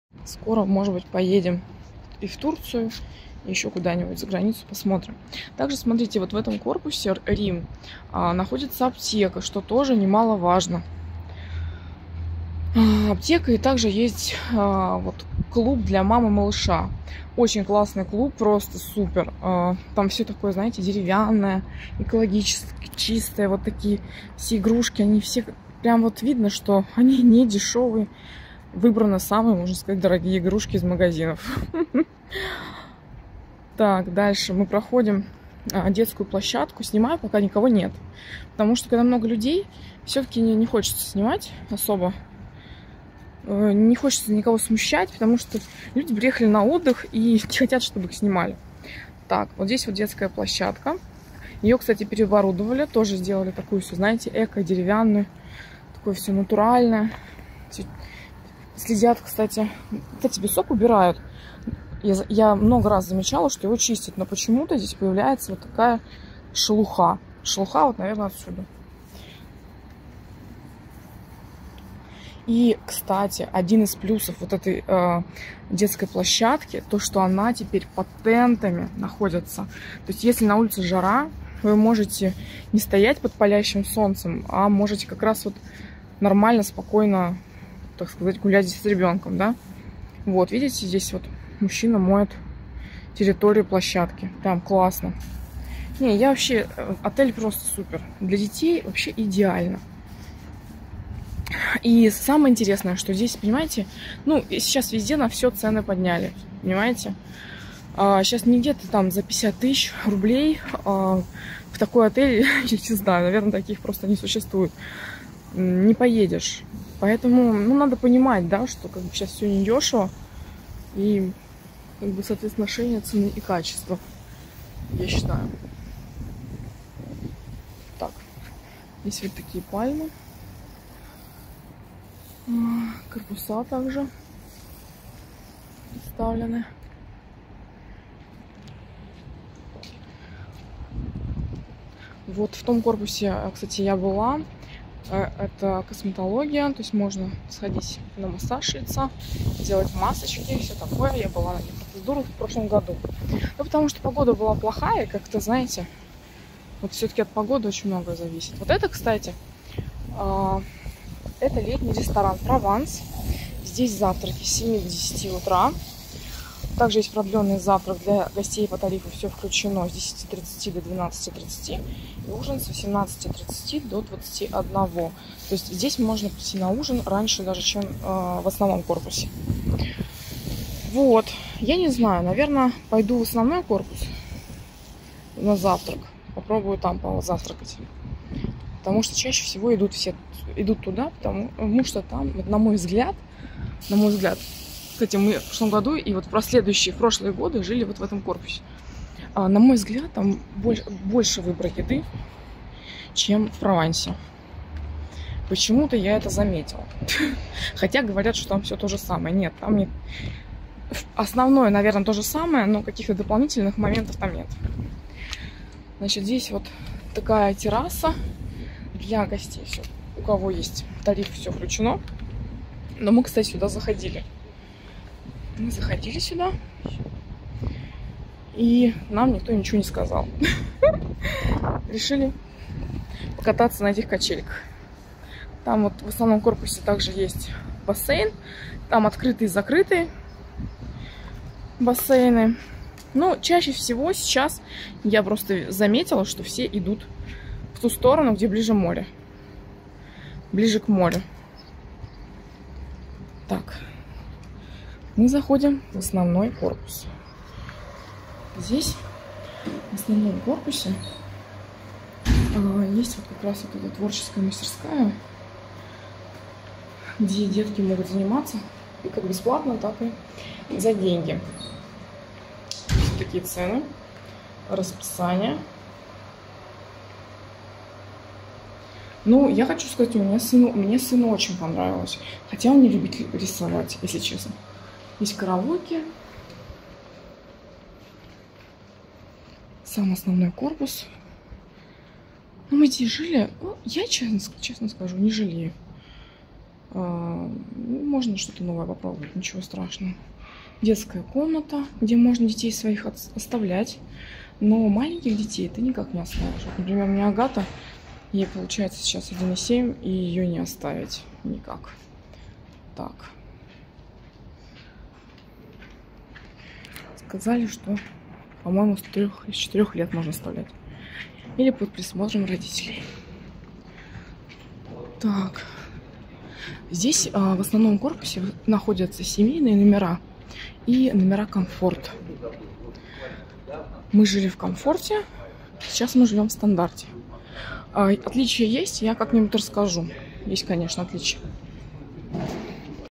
БРИДЖ РЕЗОРТ 2023. ОБЗОР
Главный корпус комплекса символизирует собой белоснежный лайнер, который бороздит просторы водной стихии, путешествуя от континента к континенту, от страны к стране. Мост – главный символ комплекса, связывающий между собой все здания и дарящий гостям возможность путешествовать по разным уголкам планеты, не покидая территории Bridge Resort. Одиннадцать стран представлены в тематических корпусах-городах Bridge Resort – Вена, Сидней, Париж, Нью-Йорк, Стамбул, Лондон, Афины, Рим, Пекин, Токио, Рио-де-Жанейро. Мы жили в Стамбуле